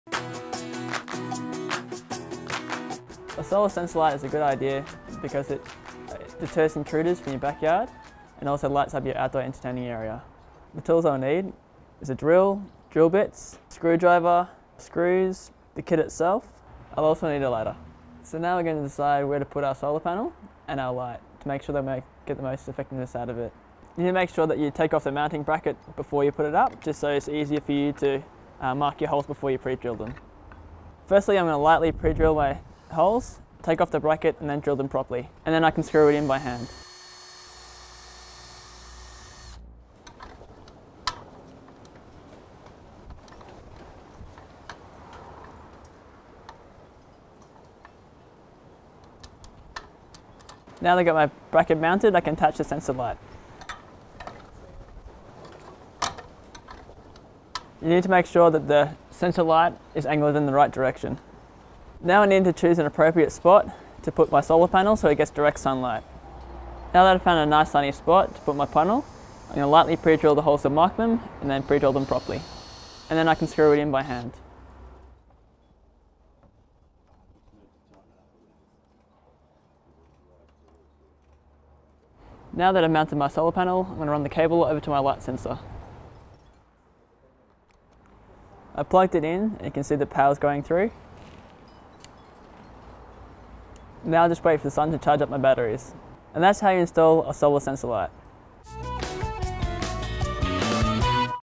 How To Install A Solar Sensor Light - DIY At Bunnings
Solar sensor lights are a great idea for deterring intruders and lighting up your entertaining area. We’ll show you how to install the light and the solar panel in just a few steps.